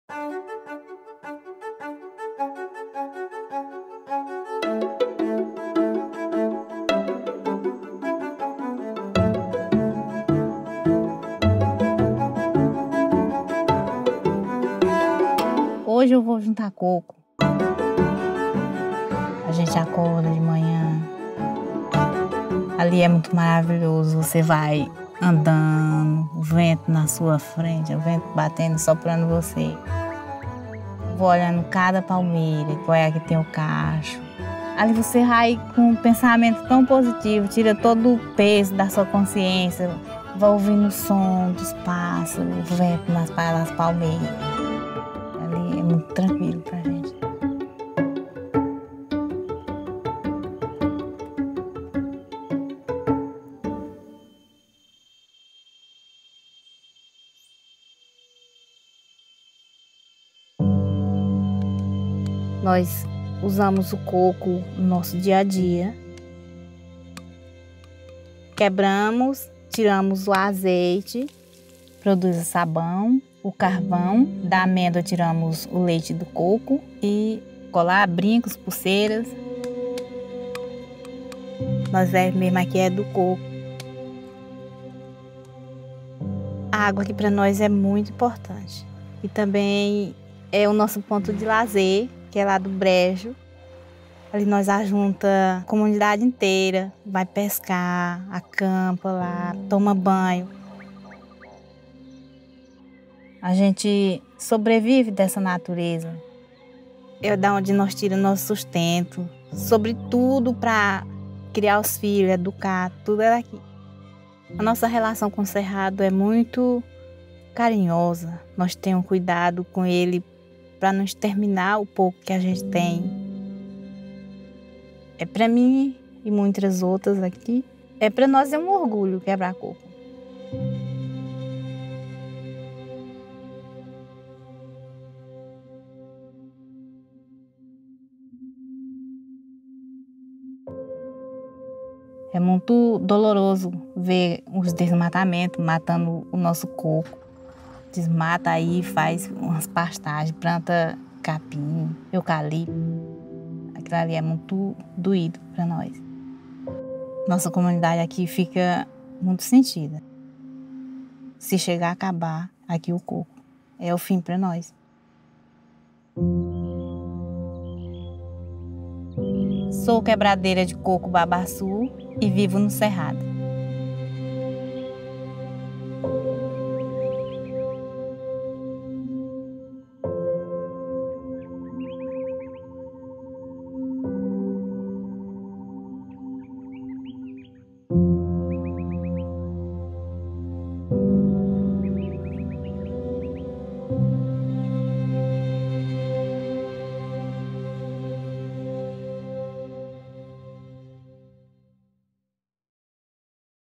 Sans savane pas de vie ! Une cueilleuse de noix de babassu témoigne
Au Brésil, la savane du Cerrado est habitée par plus de 80 peuples autochtones et communautés rurales, dont le mode de vie est aujourd'hui menacé par la déforestation.

Maria est membre d'une communauté de Quebradeiras, qui tirent leur subsistance de la cueillette des noix de babassu dans la savane du Cerrado. Elle plaide pour la protection de la savane.

Soutenons les gardien∙ne∙s de l’eau et de la biodiversité du Cerrado, en demandant aux député∙e∙s brésilien∙ne∙s d'accorder à leur territoire le statut de patrimoine national.
Un statut dont l'Amazonie, le Pantanal et la forêt atlantique bénéficient depuis 1989 !